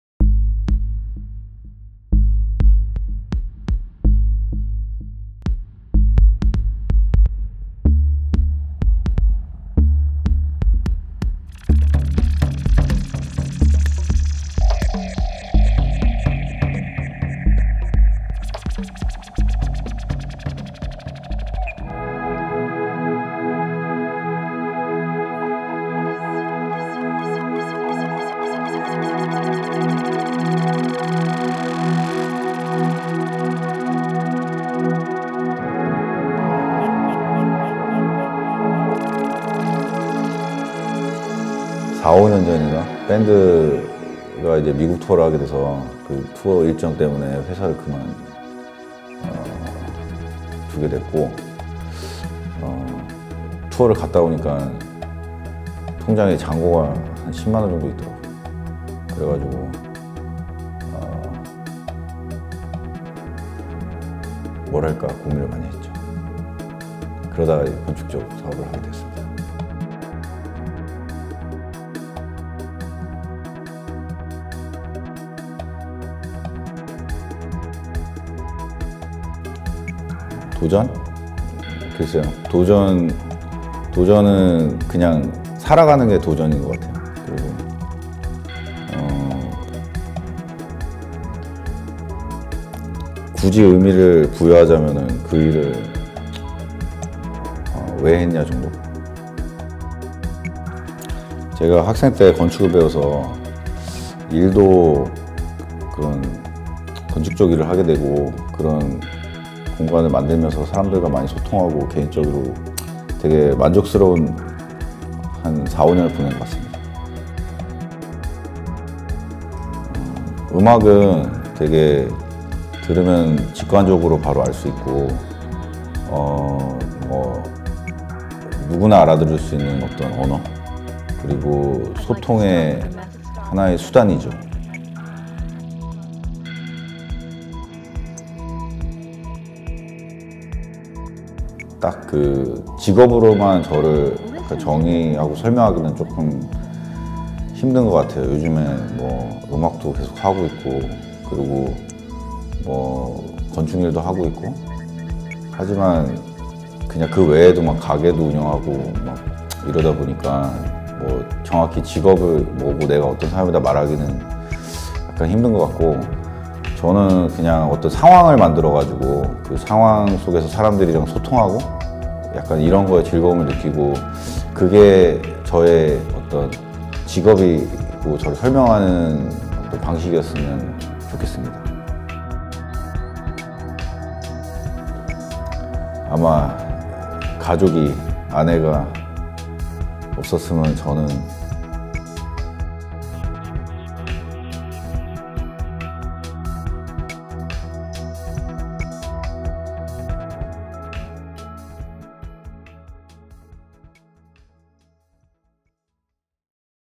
PEOPLE EP. 4 진정한 아티스트란 무엇인가?
건축가 & 뮤지션 황현진

익선동 ‘식물’, 연남동 ‘ep café & bar’,
문래동 ‘비닐하우스’, 익선동 ‘거북이 슈퍼’을 건축 디자인한 아티스트!
Architecture and Musician
그의 정체성! 아이덴티티는 과연 무엇인가?!